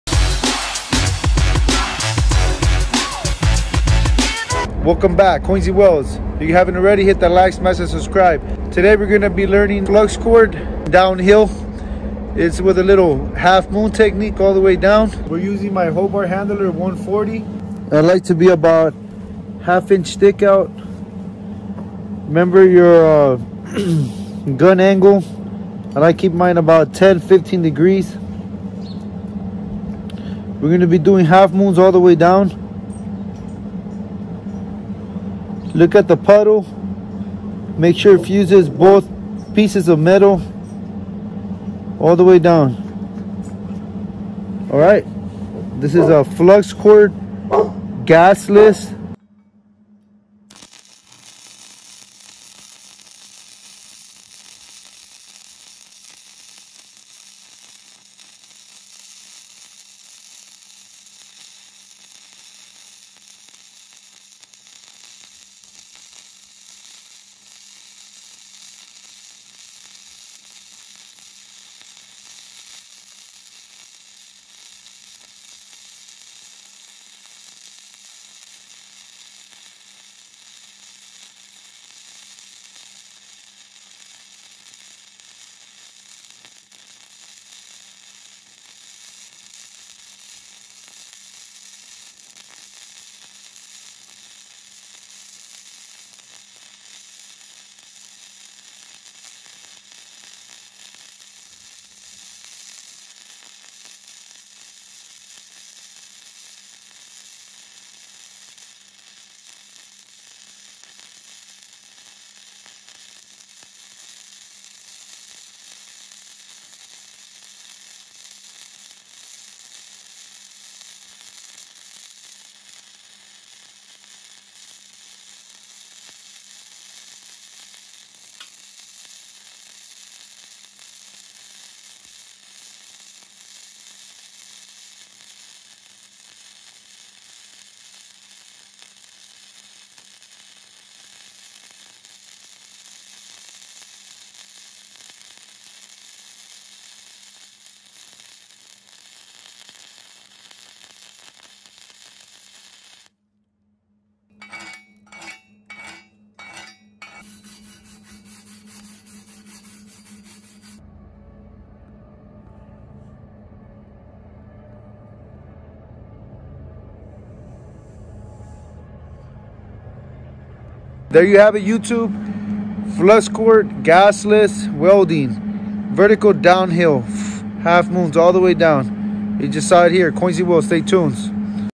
Fluxcore welding basics for beginners downhill
Fluxcore welding basics for beginners downhill is a good welding video just be sure the puddle touches both pieces of metal and you’ll be fine